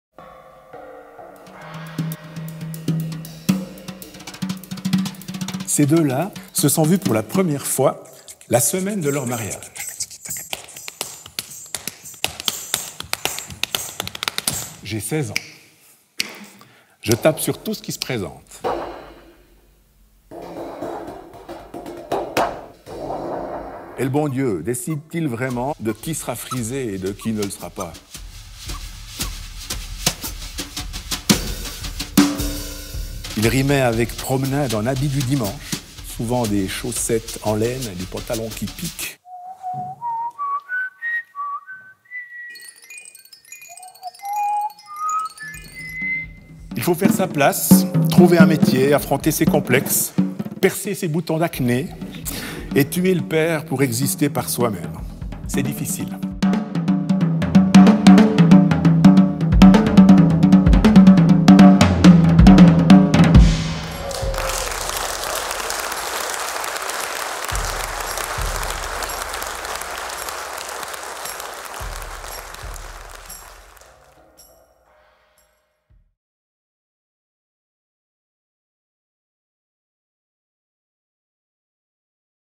" FABULARIUM... in secreto " - trailer (Alain Tissot)
Spectacle solo du percussionniste, compositeur et arrangeur suisse Alain Tissot, « FABULARIUM... in secreto » est une confidence. Alain Tissot partage son univers intérieur au-delà de ce que nous pouvons percevoir habituellement à travers sa musique.

Le public est proche, en confidence
Le lieu paisible et chaleureux
L’instrument quitte peu à peu son territoire de prédilection et
les clichés qui l’accompagnent
Le discours est transparent, épuré, musical, verbal aussi
De musique en tranches de vie, d’anecdotes en vibrations
Ainsi se partage l’histoire, en toute intimité… « in secreto »